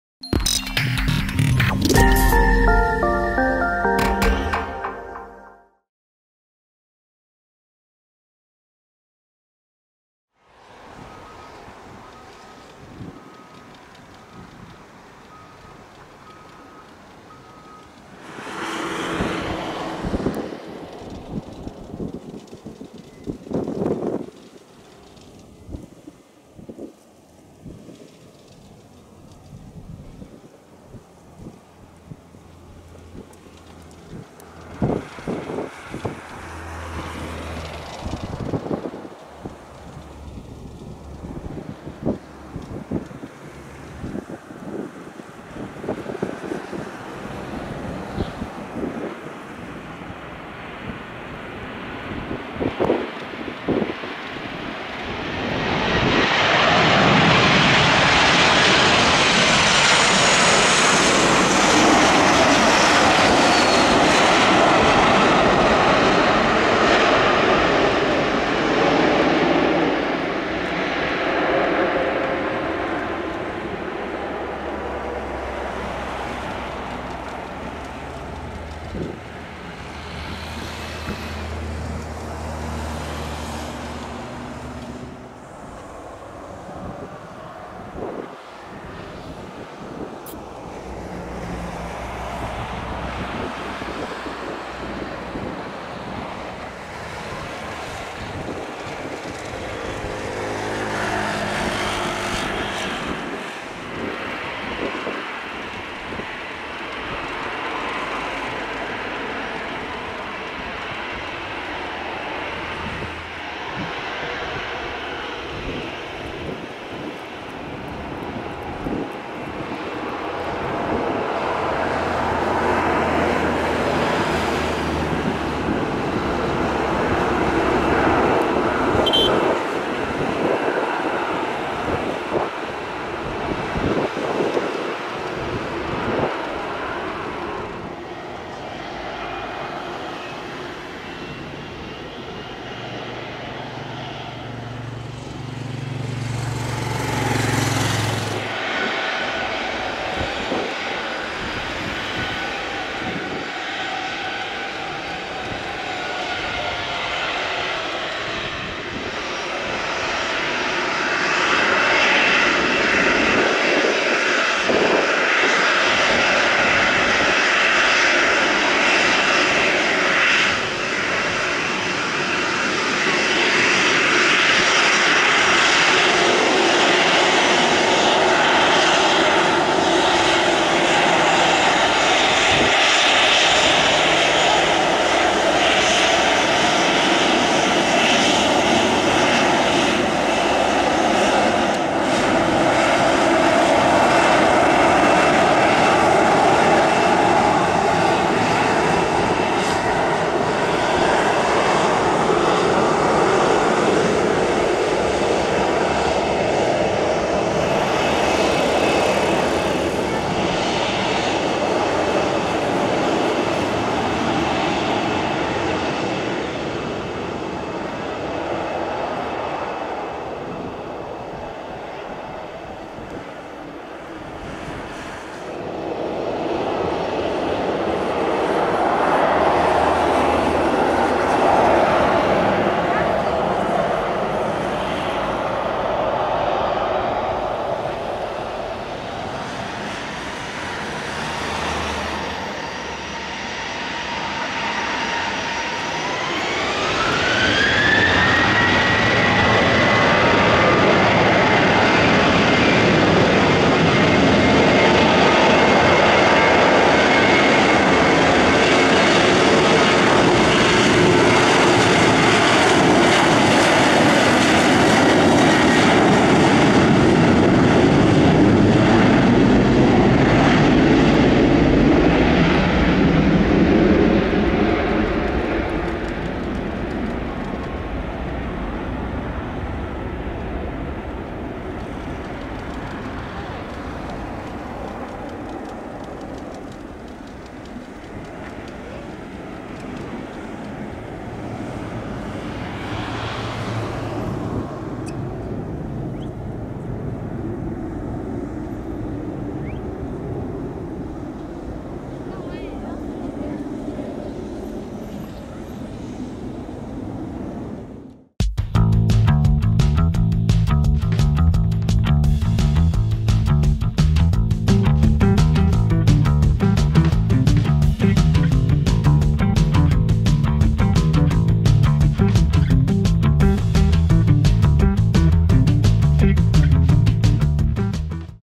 TUI BLUE LIVERY Boeing 737-800 with my new camera
This is my first video with my brand new camera, the Sony AX53, handheld, and just started to learn how to use it!

The plane is a Boeing 737-800 operated by TUI Fly hosting the TUI BLUE livery. The flight X32838 arrived from Dusseldorf and the flight X32839 departed to the same city.

Sorry for the not perfect footage, I promise to get used to the new camera in the following videos!